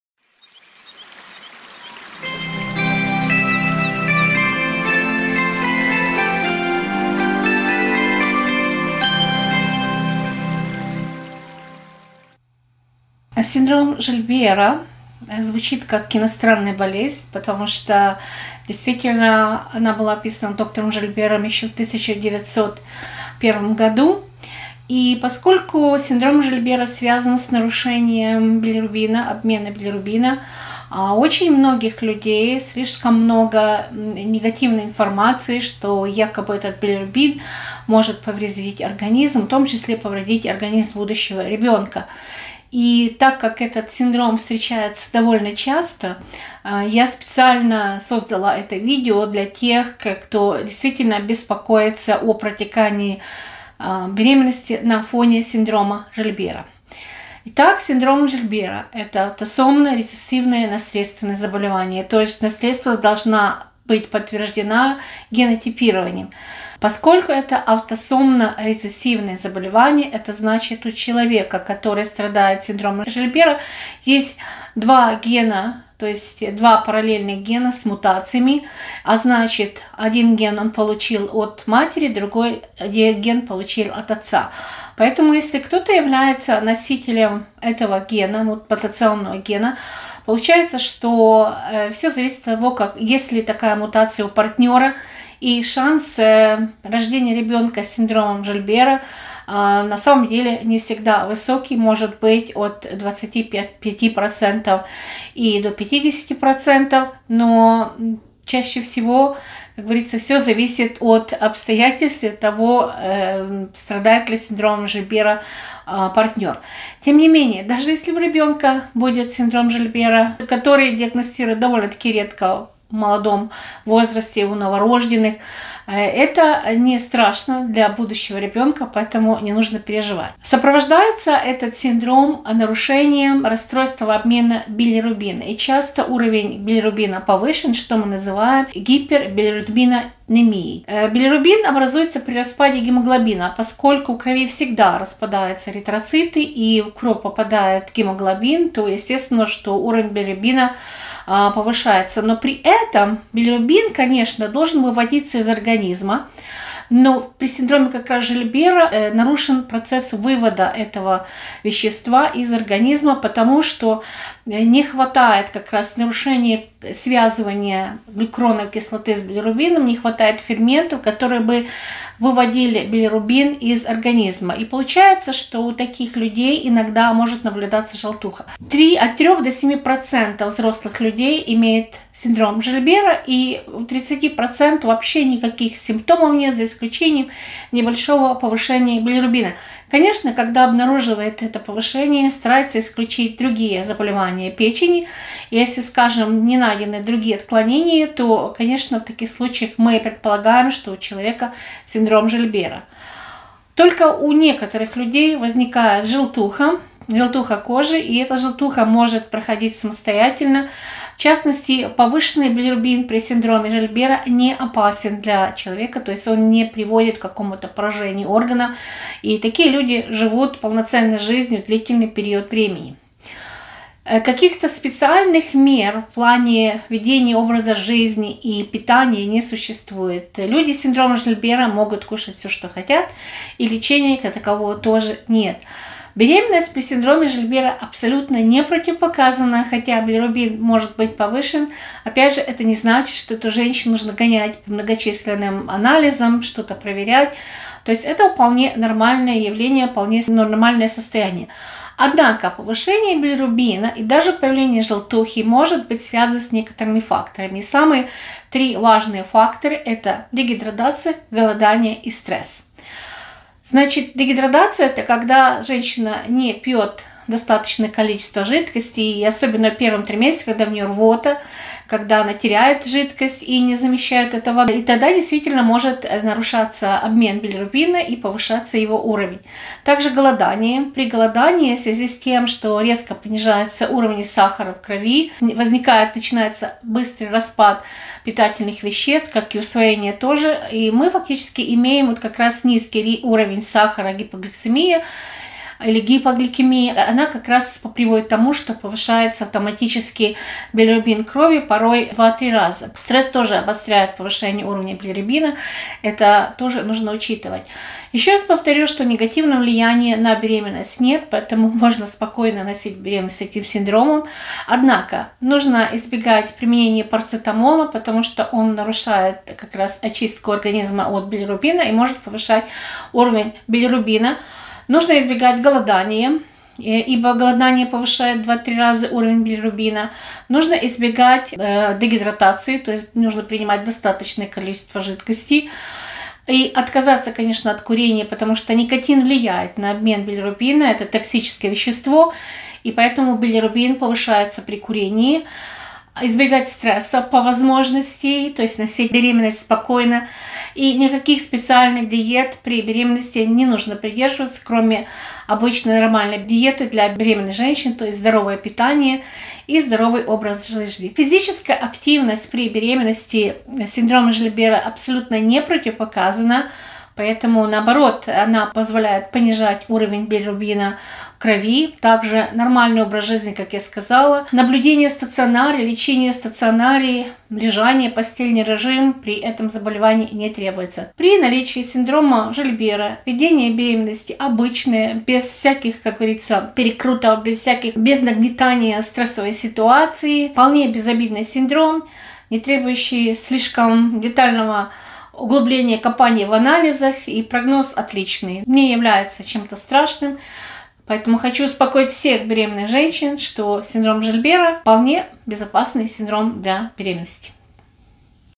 СИНДРОМ ЖИЛЬБЕРА И БЕРЕМЕННОСТЬ
o что такое синдром Жильбера
o как правильно диагностировать этот синдром
o влияние беременности на синдром Жильбера
o влияние синдрома Жильбера на беременность

• Art from Olena’s Heart
Всем отличного здоровья!